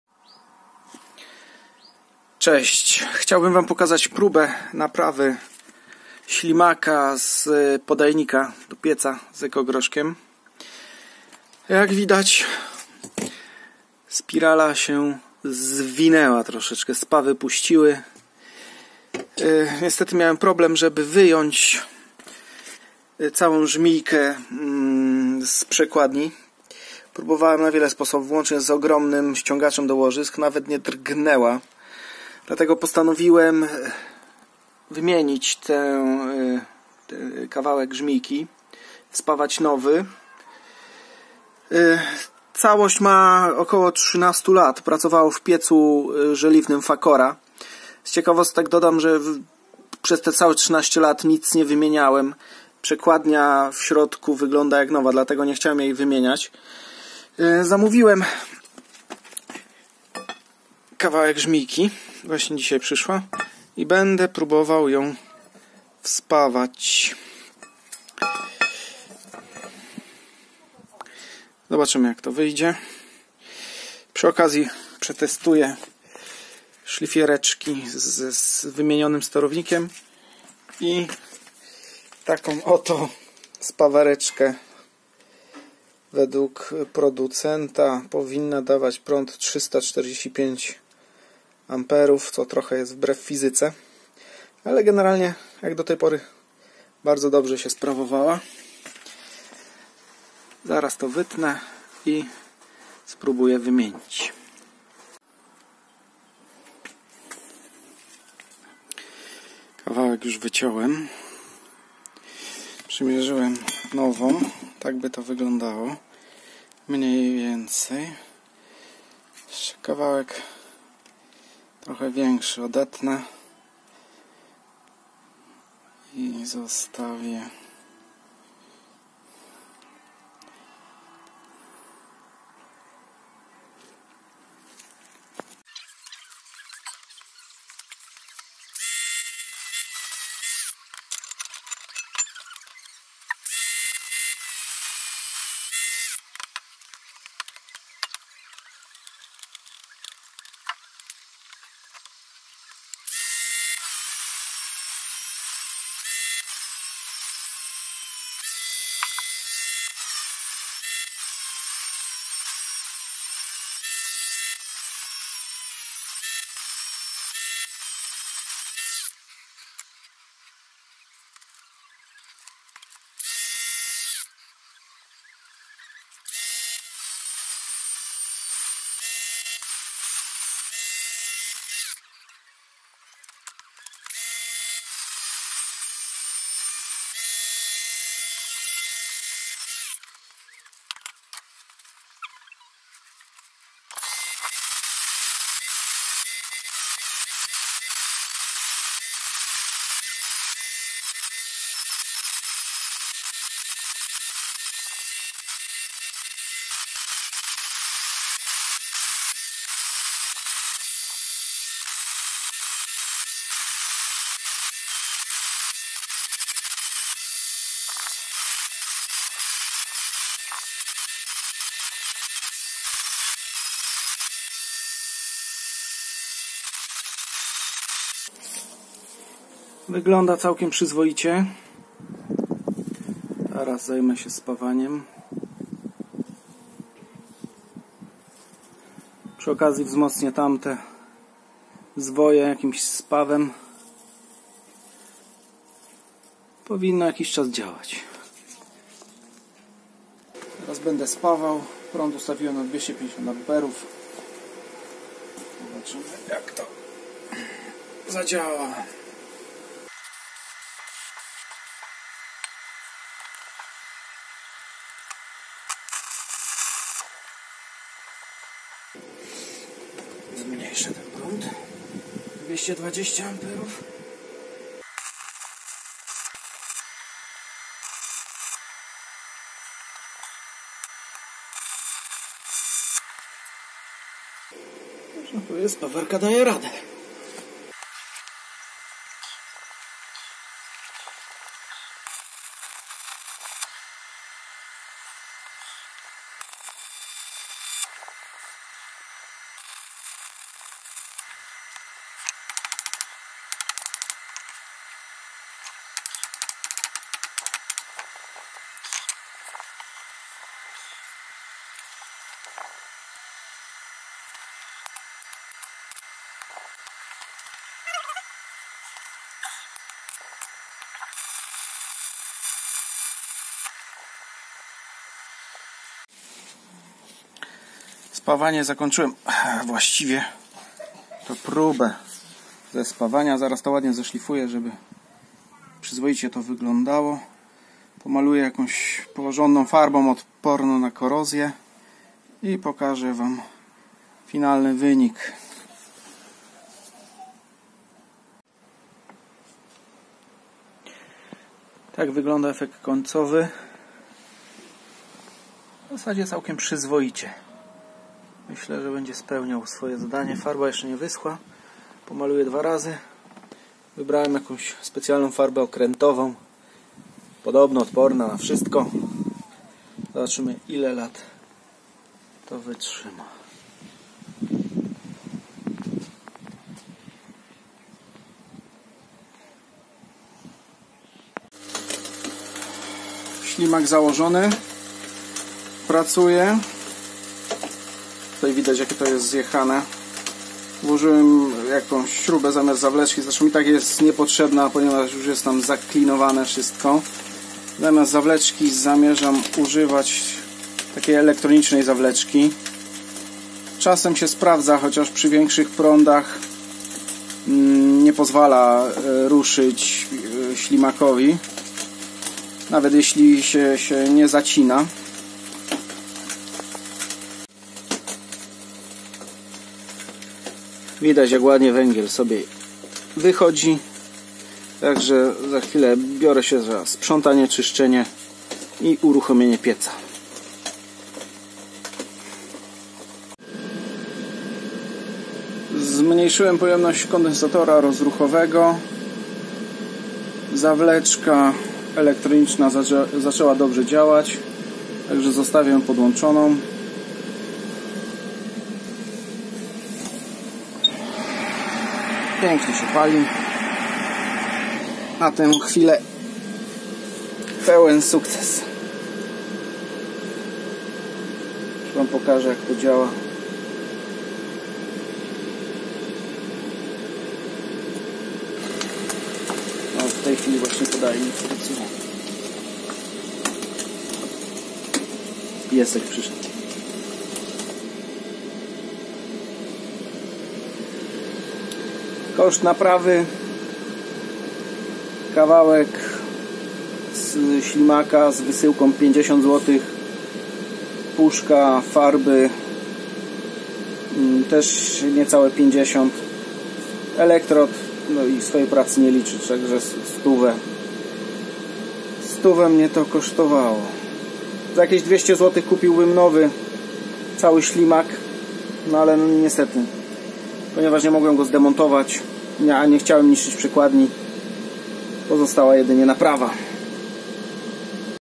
Naprawa podajnika ślimakowego w kotle na ekogroszek.
Naprawa urwanej "żmijki" w podajniku kotła na ekogroszek. Ponieważ nie mogłem zdemontować całego ślimaka, zdecydowałem się na naprawę zerwanego fragmentu. Przy okazji wykonałem mały tekścik szlifierki Parkside Performance z wymienionym sterownikiem BLDC oraz spawarki z prądem maksymalnym 345 A. W związku z awarią zawleczki zastosowałem zawleczkę elektroniczną.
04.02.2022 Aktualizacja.
Podajnik przestał działać. Nie pomogło nawet wsparcie w postaci klucza do rur i 1,5 m przedłużki. Po wyjęciu okazało się, że w palniku, z pyłu zrobił się czop o twardości betonu.
Żmijka nie poddała się obciążeniom i wygląda nadal jak w dniu naprawy!
Powodem prawdopodobnie była zła konstrukcja żmijki. Mogłaby być dłuższa oś z kontrślimakiem . Producent o tym nie pomyślał.
Może poprawię to  przy następnej naprawie.